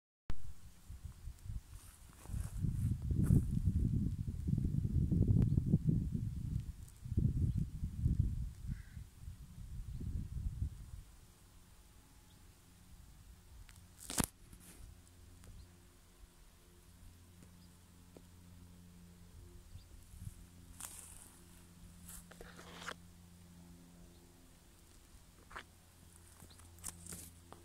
Cherry Plum Tree |Buhay ay Ganyan
Prunus cerasifera is a species of plum known by the common names cherry plum and myrobalan plum. It is native to Southeast Europe and Western Asia, and is naturalised in the British Isles and scattered locations in North America.